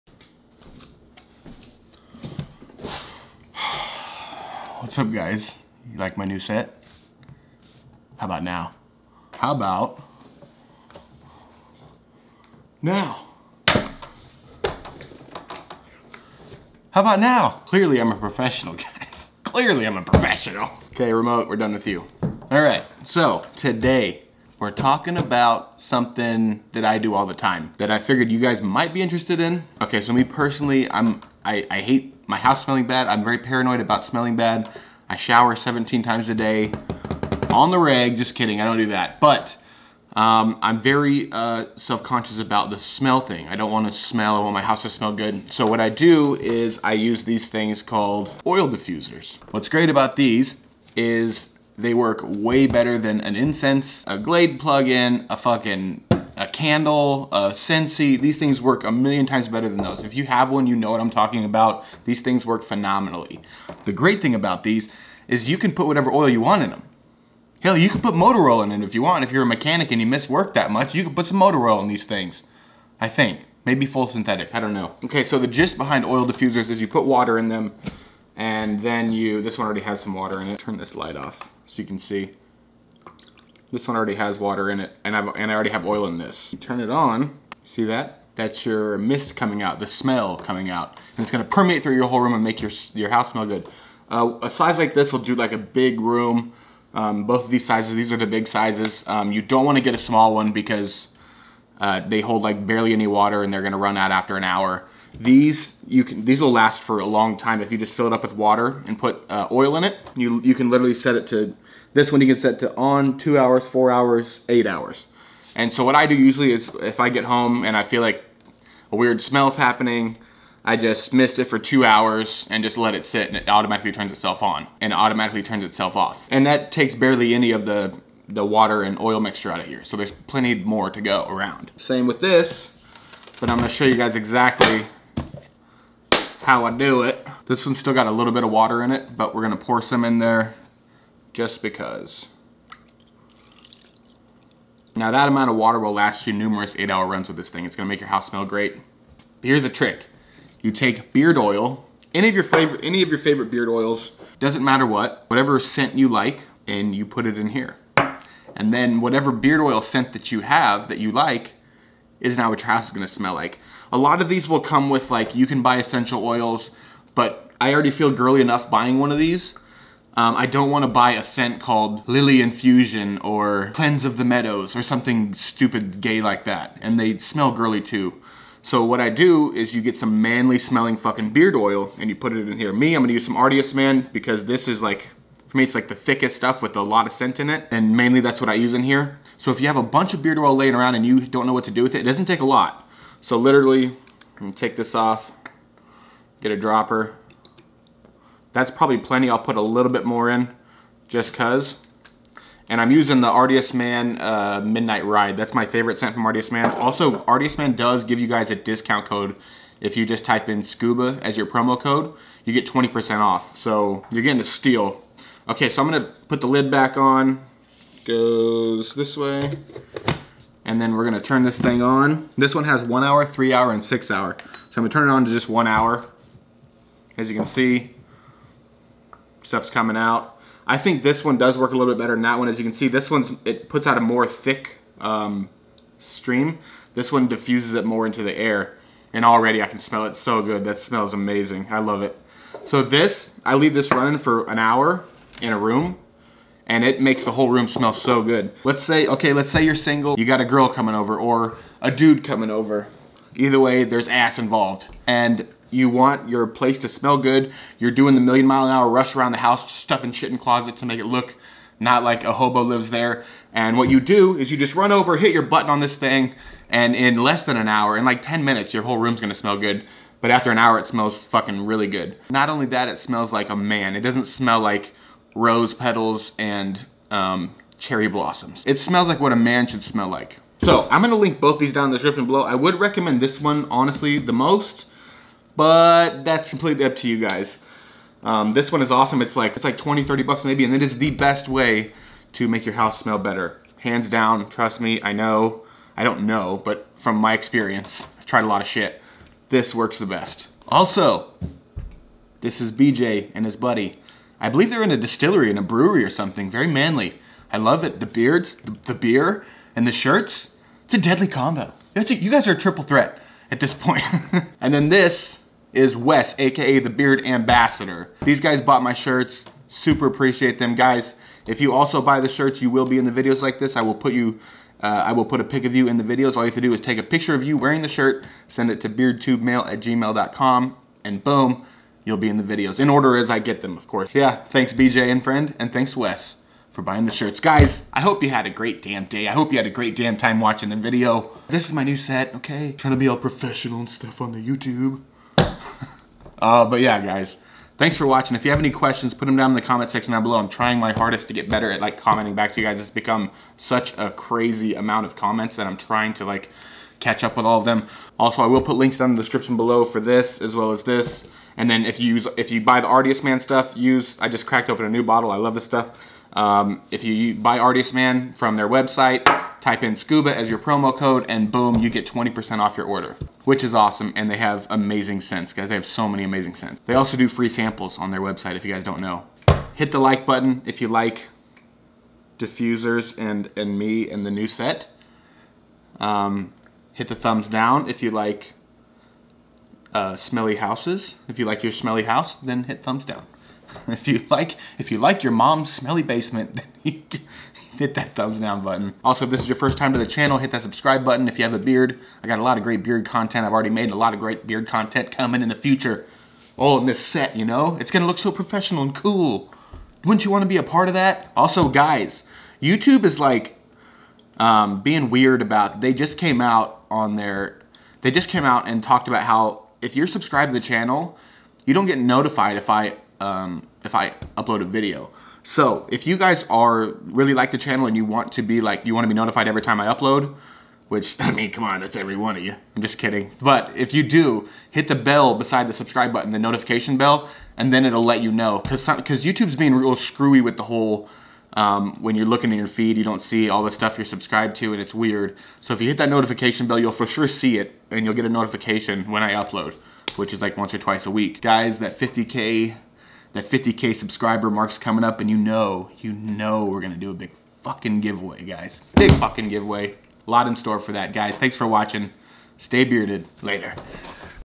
BEARD OIL HACK | Make your house smell like your favorite beard oil
This is how to use an oil diffuser to make your house smell like your favorite beard oil.
Best one I own
Second best one